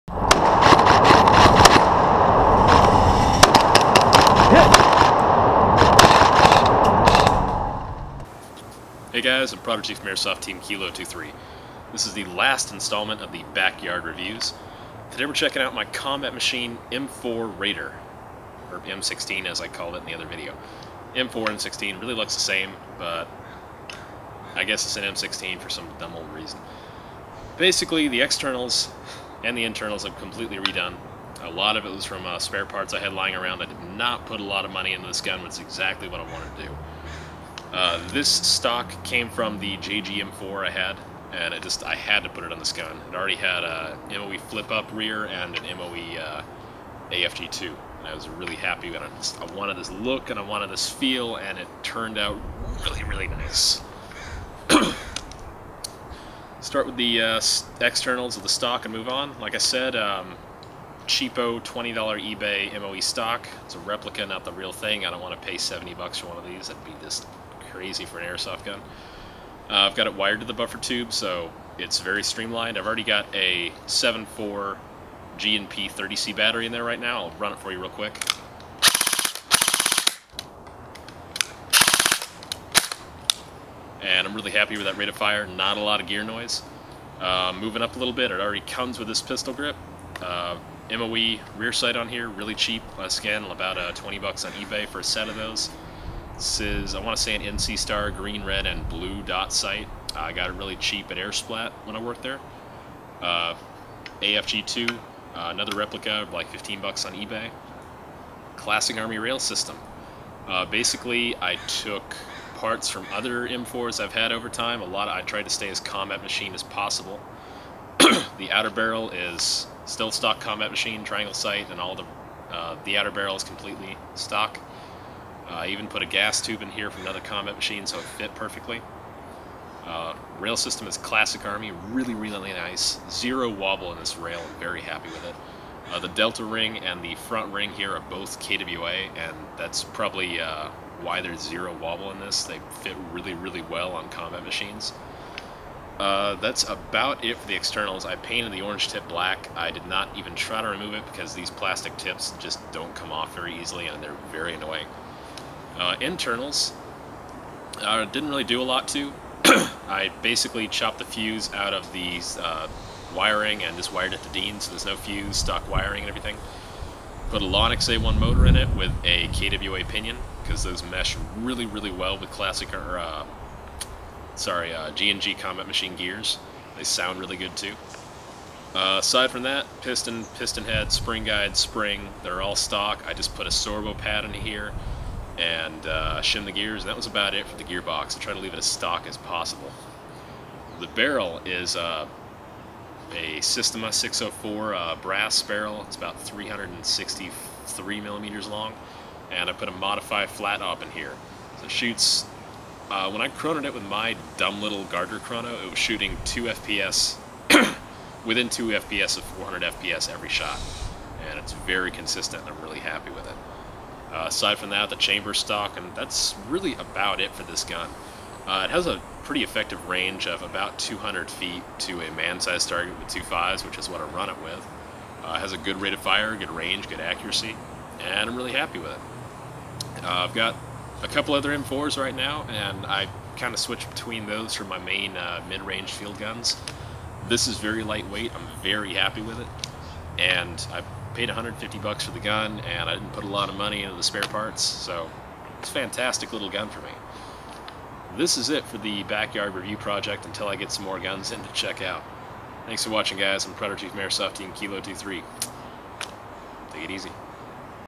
Backyard Review: G&G Combat Machine M16 (M4) Raider -ASTKilo23-
If you have been watching recent videos, you know that this gun is no longer in existence and has been turned into a Socom47. Nevertheless, I reviewed it before I did all of that work. So here it is! Enjoy!